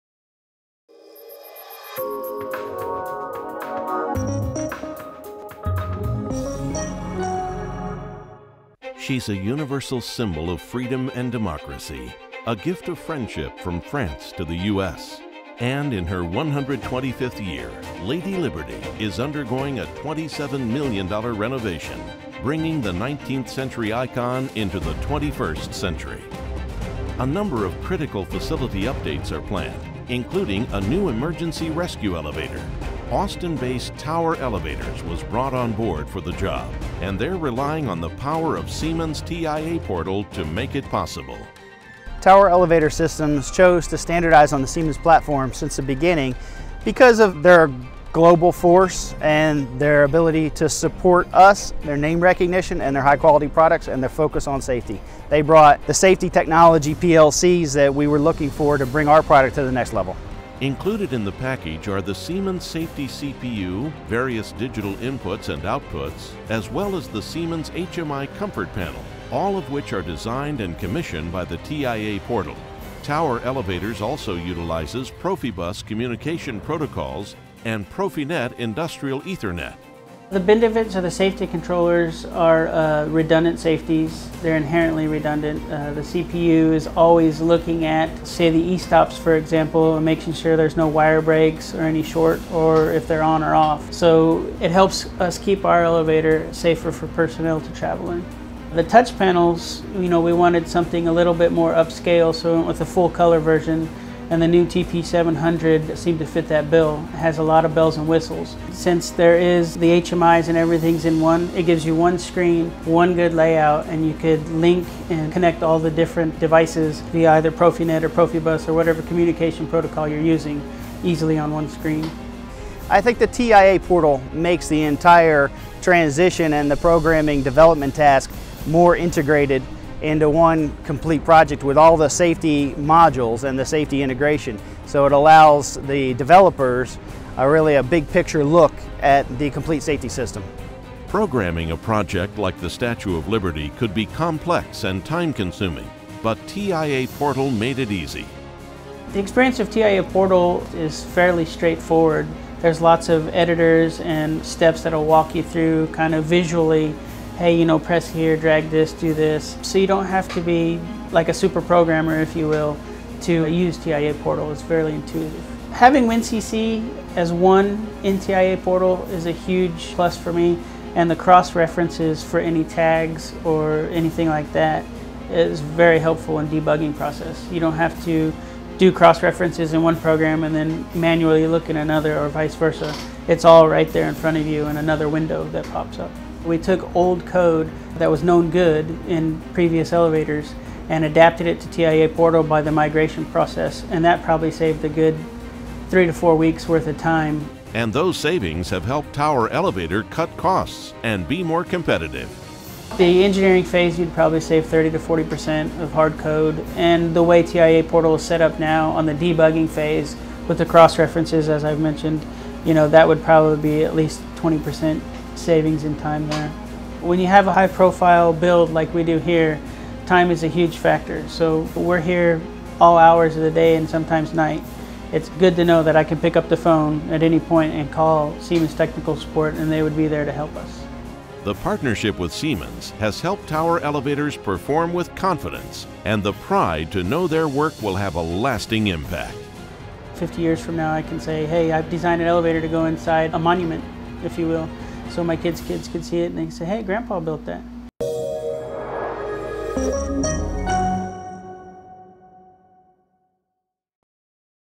Tower Elevator Systems, Inc.
Speciality elevator upgrade at the Statue of Liberty uses Siemens TIA Portal and reduces engineering and programming time 20 - 30% over previous efforts.